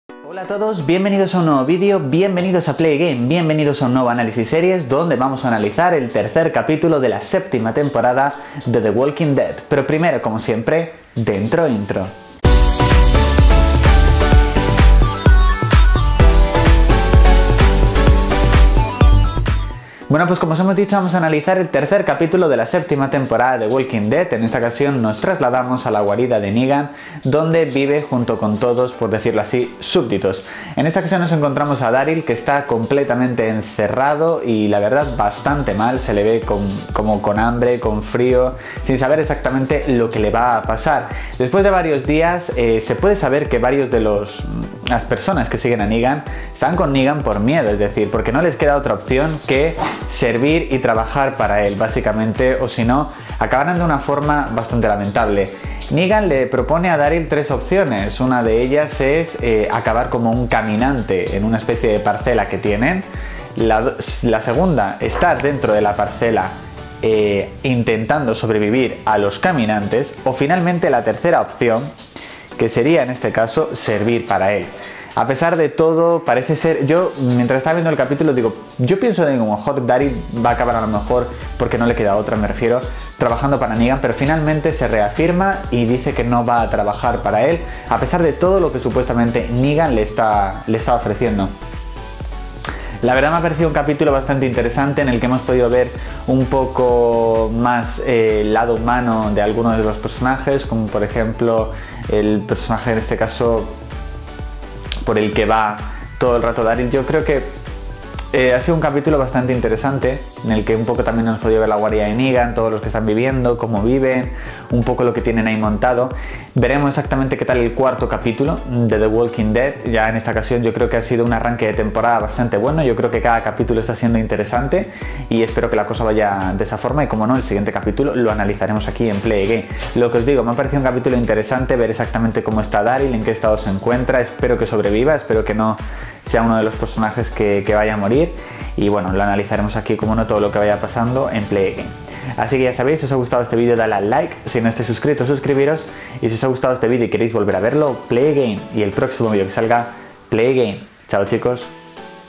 Análisis Series | The Walking Dead 7x03 Análisis a Fondo ¡Spoilers!
Nueva entrega de 'Análisis Series' donde analizamos a fondo el 7x03 de la serie 'The Walking Dead' ¡Atención Spoilers!

NUESTRAS LISTAS